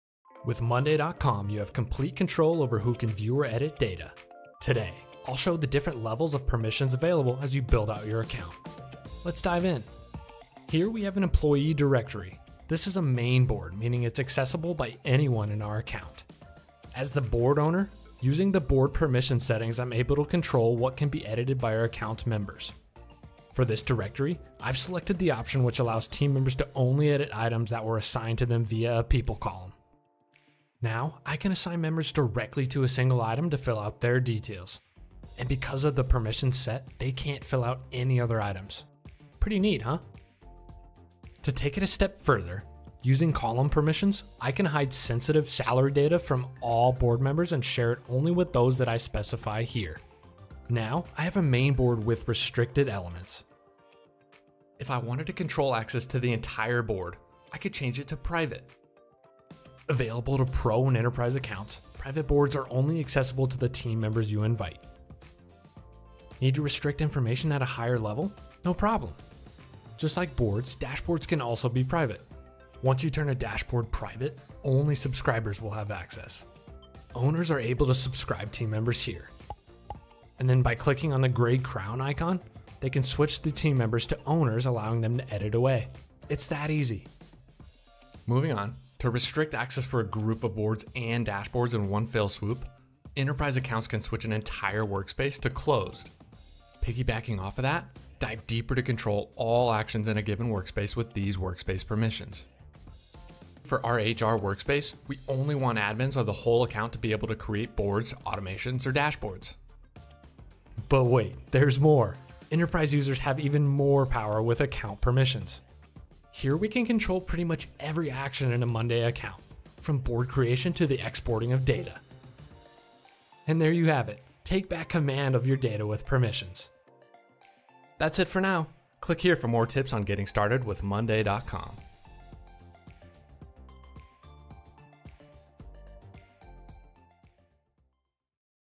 Autorisations et restrictions sur monday.com
Chez monday.com, nous voulons vous donner les moyens de restreindre la consultation ou la modification des données à ceux qui elles sont destinées. C'est pourquoi nous proposons plusieurs types d'autorisations personnalisables, parmi lesquelles :

Autorisations de tableau (types et restrictions)
Autorisations de colonnes
Autorisations de tableau de bord (types et restrictions)
Autorisations de l'espace de travail (types et restrictions)
Autorisations du compte